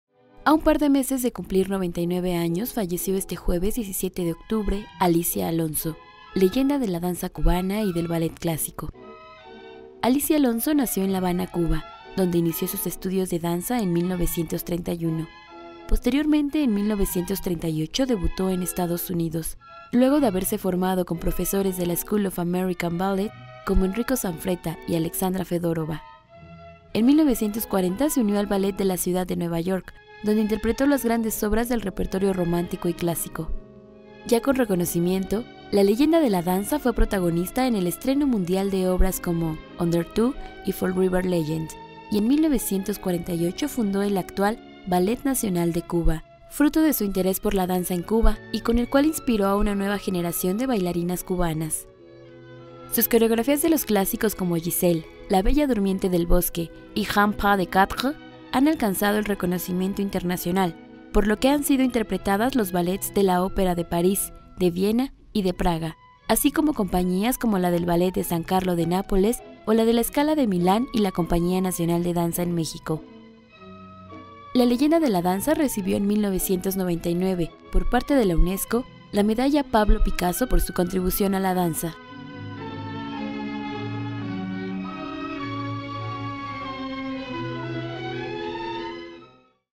Fallece Alicia Alonso, leyenda mundial de la danza
A un par de meses de cumplir 99 años, falleció este jueves, Alicia Alonso, leyenda de la danza cubana y del ballet clásico.